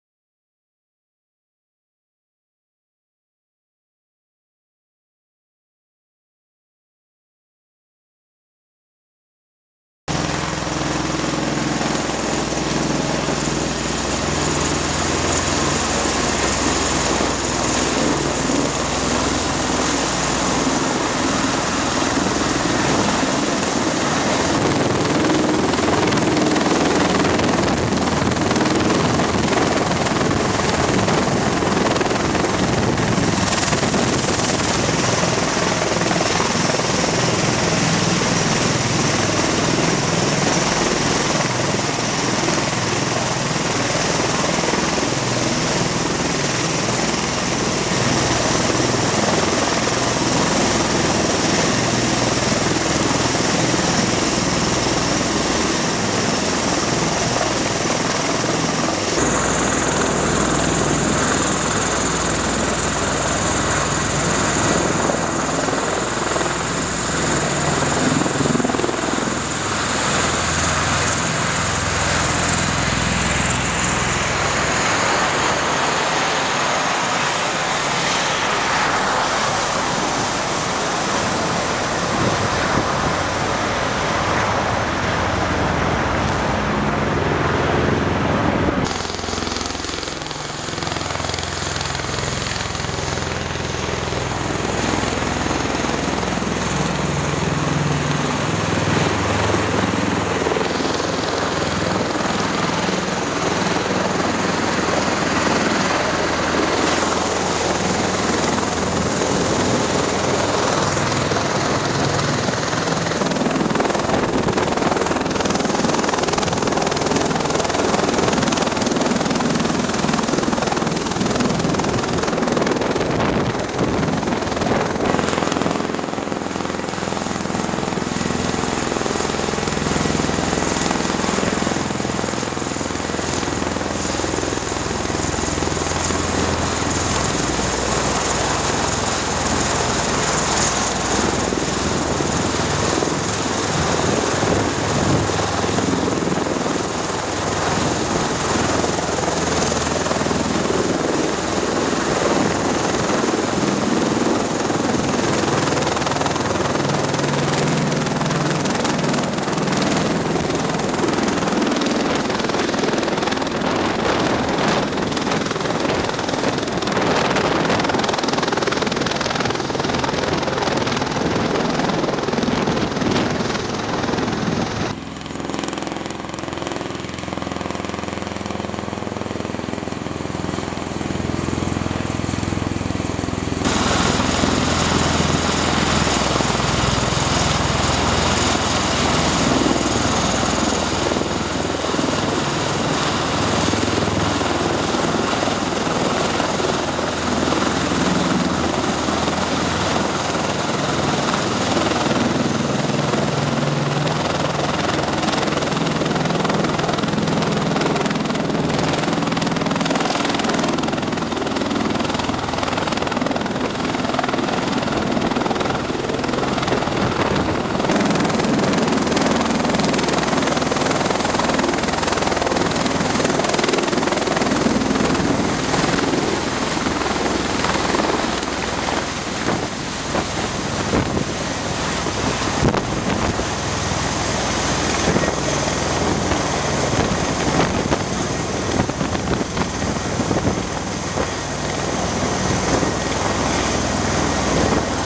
MV-22B Osprey Touch-and-Go Landings on USS Bataan
26th MEU/USS Bataan Group Sail Exercise.  Video by Cpl. Chris Stone | 26th Marine Expeditionary Unit | Date: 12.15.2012.  MV-22B Ospreys assigned to Marine Medium Tiltrotor Squadron (VMM) 266 Reinforced, 26th Marine Expeditionary Unit (MEU) conduct touch and go landings on the flight deck of the USS Bataan (LHD 5) off the coast of North Carolina, Dec. 15, 2012. The landings were a part of the unit's required qualifications for deployment. The 26th MEU operates continuously across the globe, providing the president and unified combatant commanders with a forward-deployed, sea-based quick reaction force. The MEU is a Marine Air-Ground Task Force capable of conducting amphibious operations, crisis response, and limited contingency operations. The 26th MEU is slated to deploy in 2013. (U.S. Marine Corps motion media by Cpl. Christopher Stone/Released).  HD Video.  High definition.